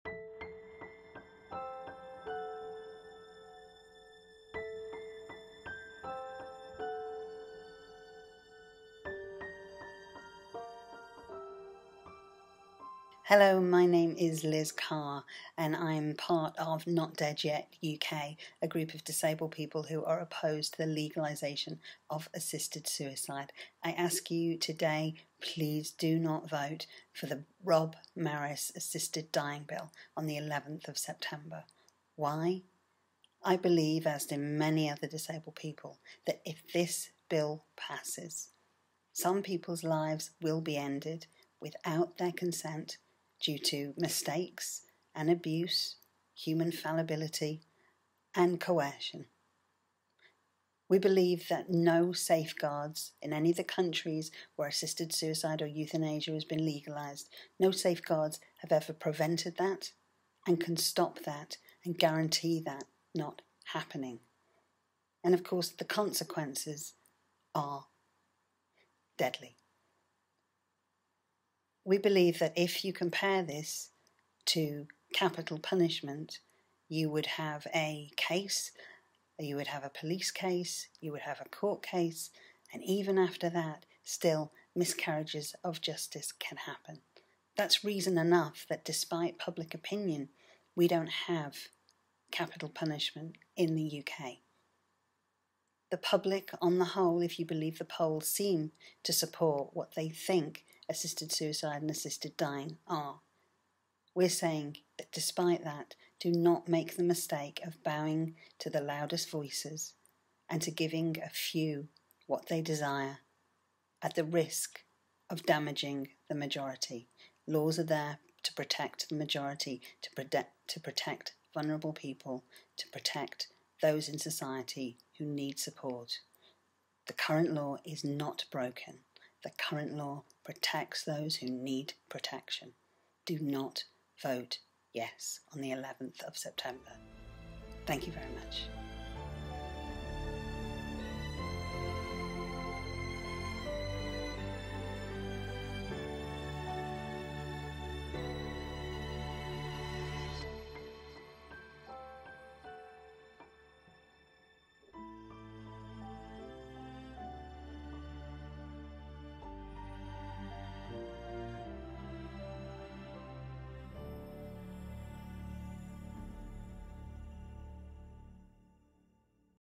Not Dead Yet UK - September 2015 - Liz Carr
Not Dead Yet UK is a collective of disabled people from all over the UK who have one thing in common - we ALL strongly oppose the legalisation of assisted suicide.

We all have our own individual & varied reasons but we agree that the Bill going through Parliament at the moment (the Assisted Dying Bill) is unsafe, unnecessary & unfair. It will immortalise in law the inequality that still exists between ill/disabled and non-disabled people.

This film is part of our September 2015 video campaign that aims to inform both MPs & the general public alike WHY we oppose moves to legalise assisted suicide.

In this video Liz Carr (Actress/Not Dead Yet UK Campaigner) talks about the dangers of the Assisted Dying Bill and why she and Not Dead Yet UK members oppose the legalisation of assisted suicide.

All videos are produced & edited by:

Lenka Cooper
Juliet Marlow
Tam Hunt